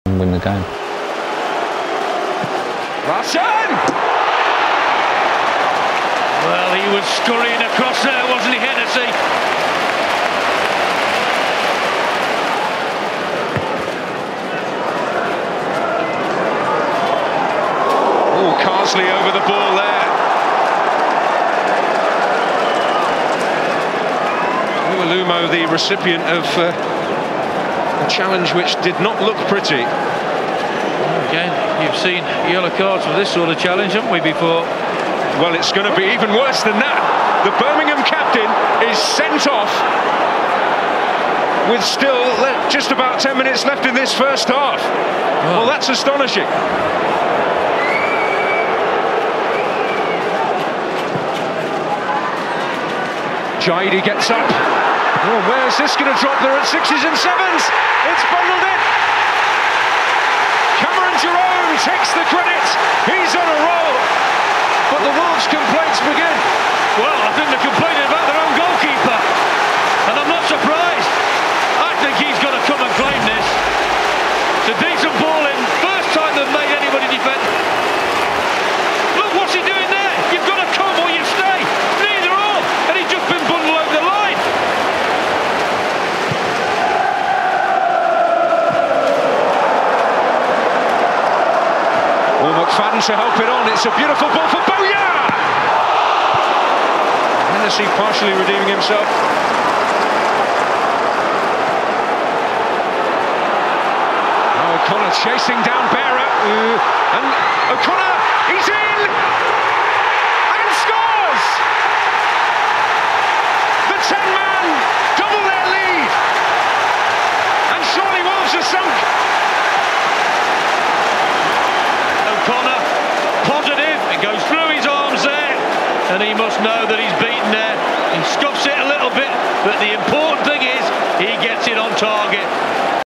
Birmingham City 2-0 Wolverhampton Wanderers 2008/2009
Blues beating Midlands rivals Wolves on course for automatic promotion for both clubs. Copyright of Sky Sports and PDI Media.